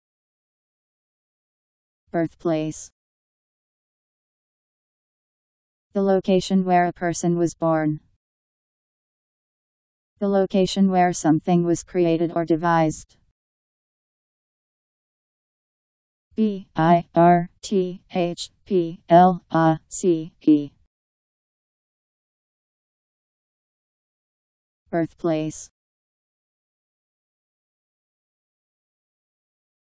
Birthplace Meaning
Video shows what birthplace means. The location where a person was born.. The location where something was created or devised..  Birthplace Meaning. How to pronounce, definition audio dictionary. How to say birthplace. Powered by MaryTTS, Wiktionary